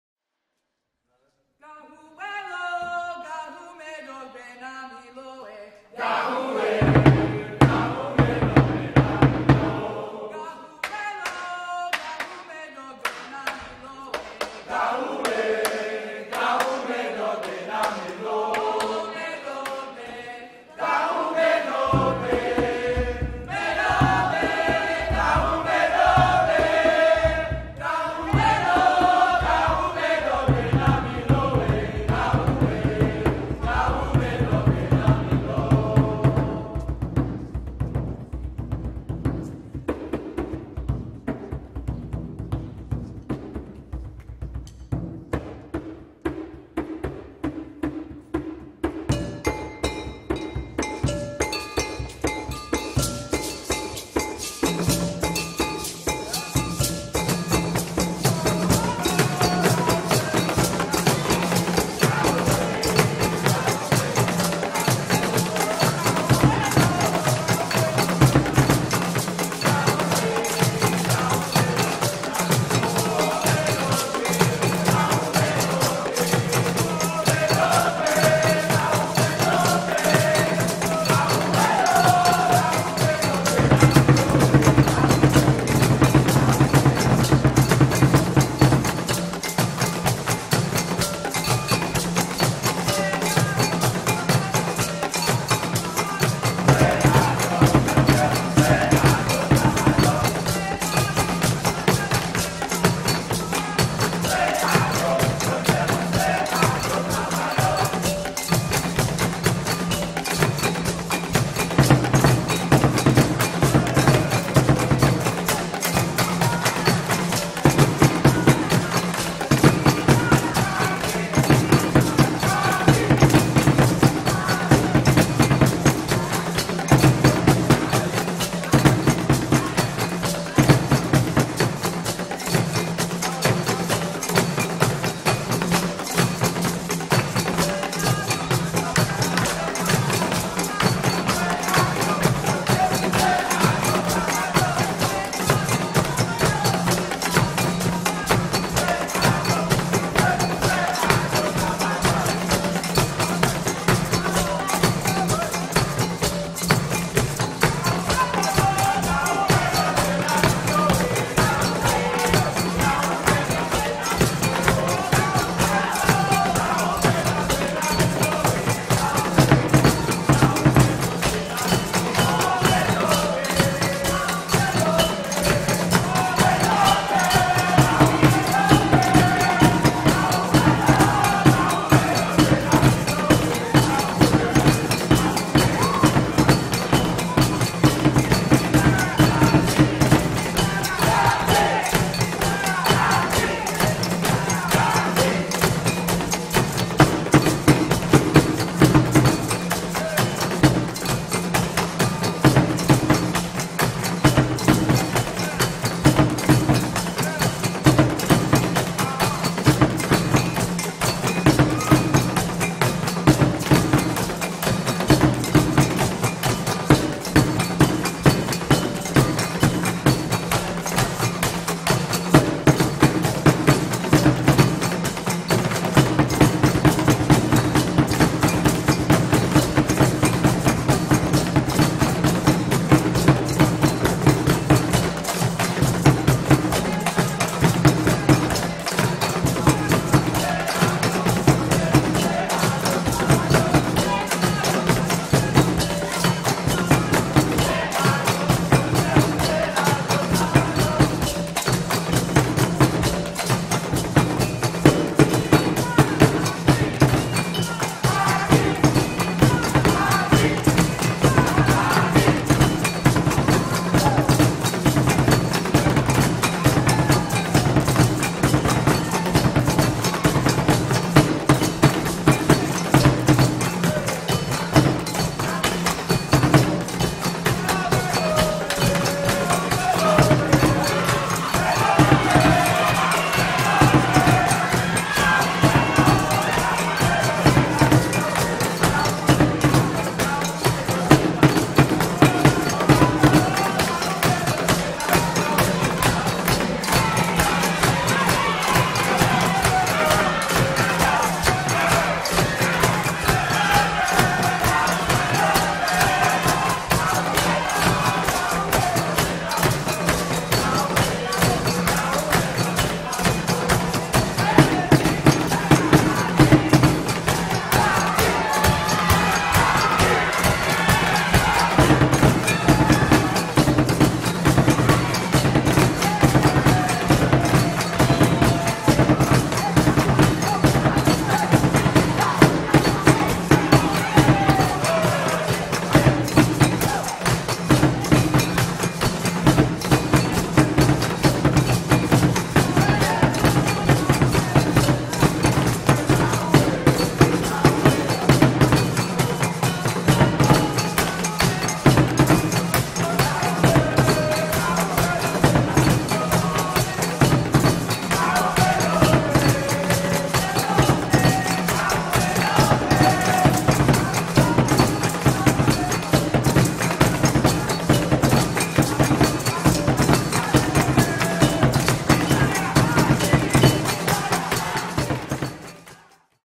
Gahu Dance-Drumming
Gahu dance-drumming from Ghana performed by students in the Crane West African Ensemble in 2017 with master drummer Kwaku Kwaakye Obeng.